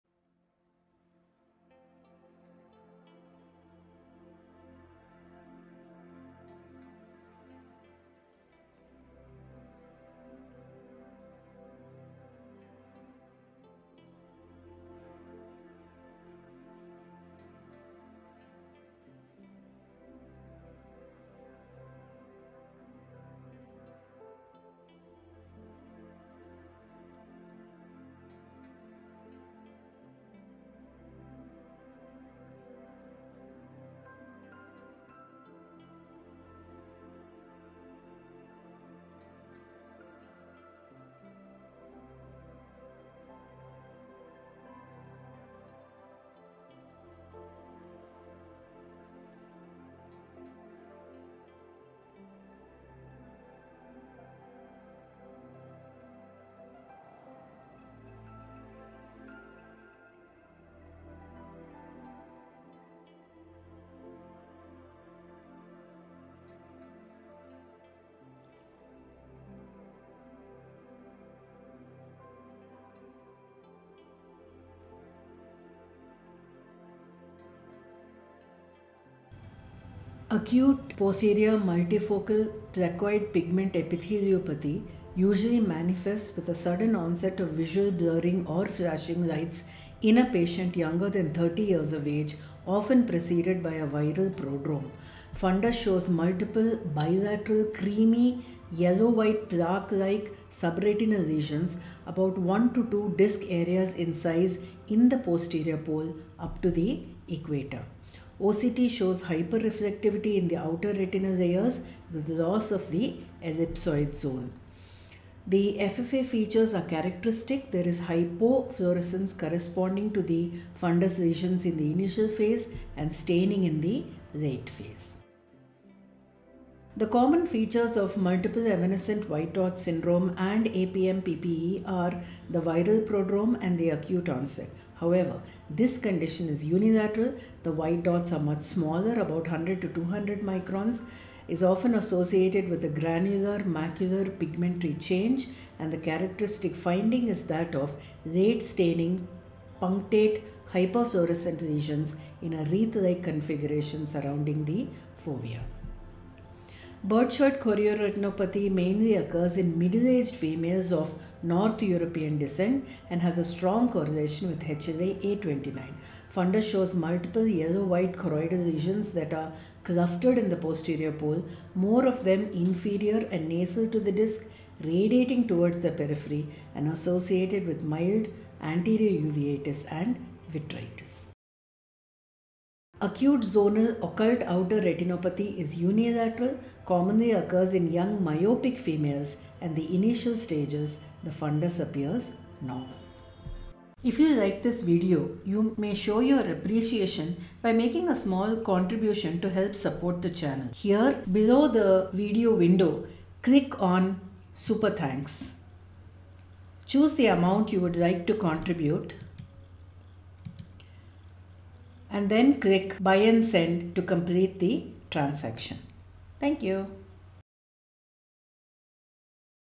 AP's Image Quiz 1
A multiple choice quiz for you to diagnose from the fundus picture, OCT, and fluorescein angiogram. A brief explanation is given at the end.